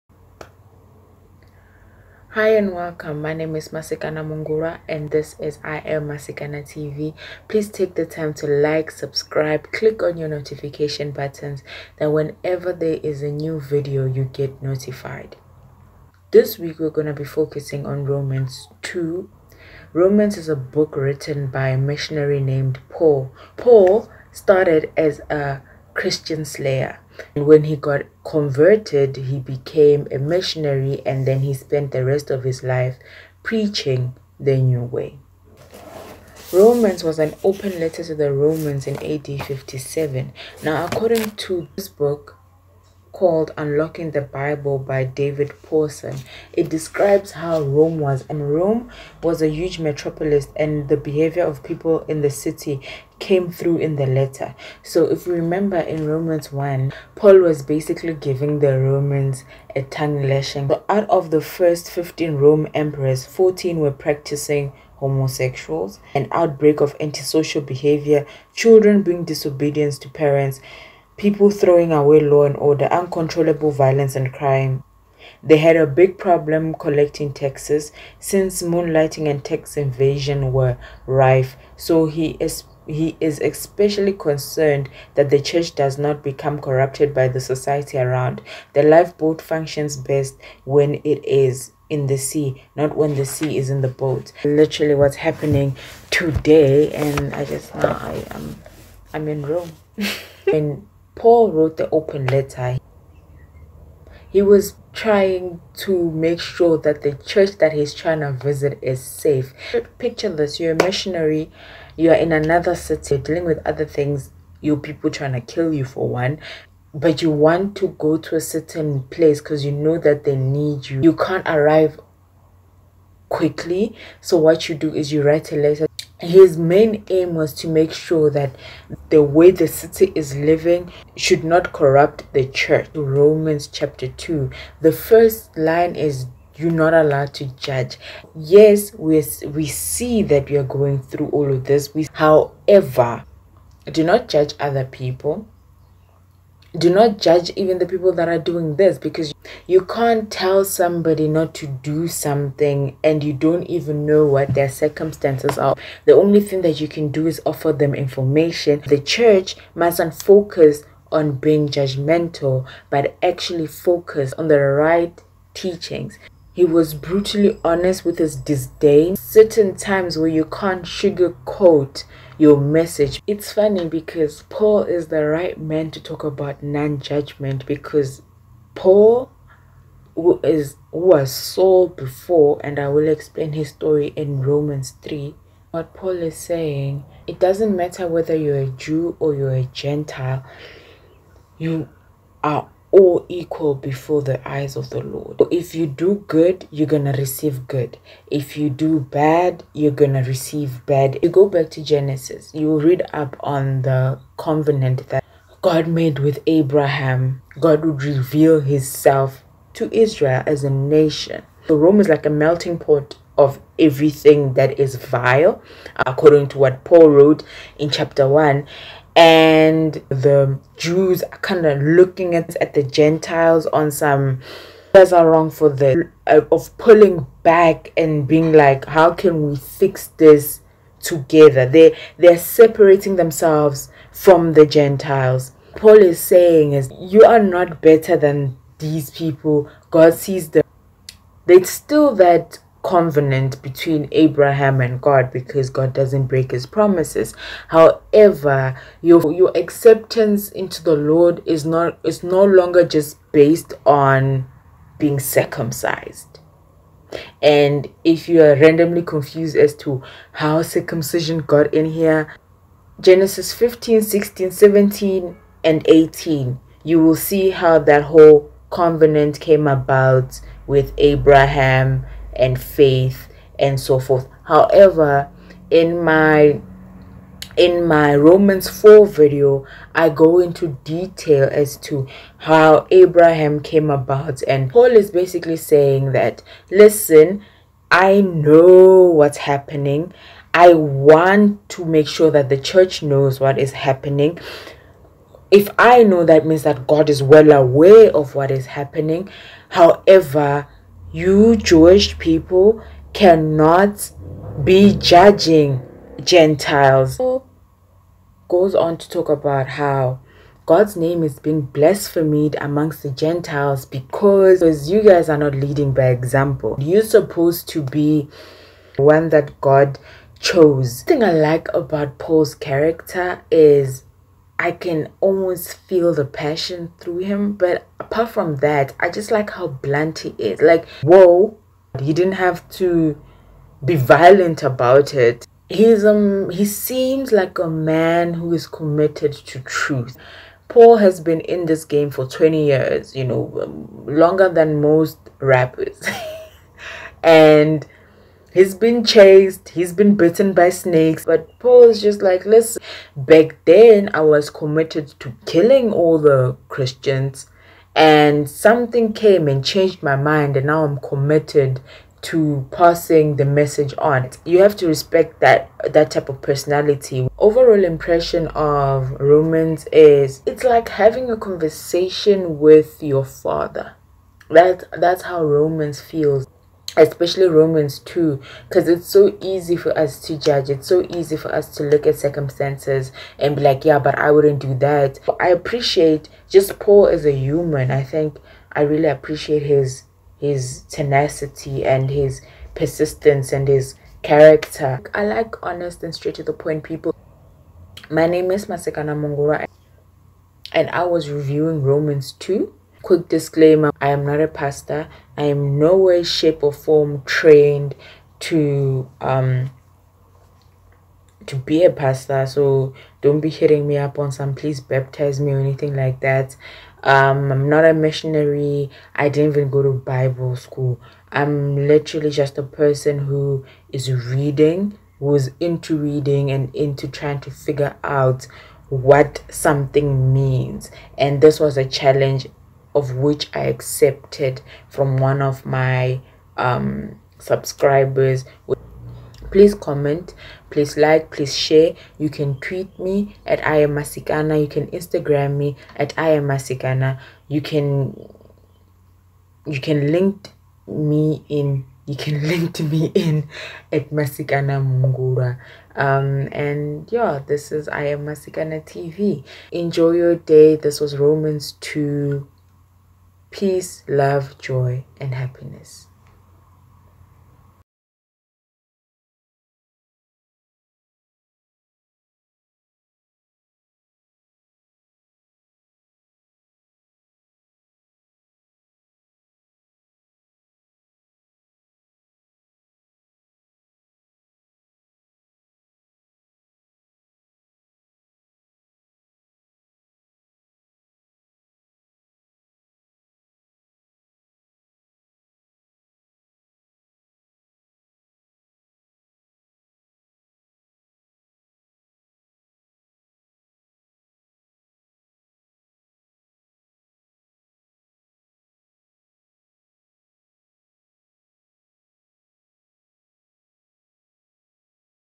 Thou Shall Not Judge Romans 2 Review
Want an in-depth look at the Biblical open Letter Written by Paul to the people of Rome? This Bible Breakdown Series will break down every chapter of the book of Romans 1- 16. The Romans 2 review consists of; Pauls's background,  intentions of the open letter, and the lessons we can use for modern-day life. Romans chapter 2 mains theme is Thou shall not judge.

Video  narrated by Masikana Mungulwa.

Unlocking the Bible by David Pawson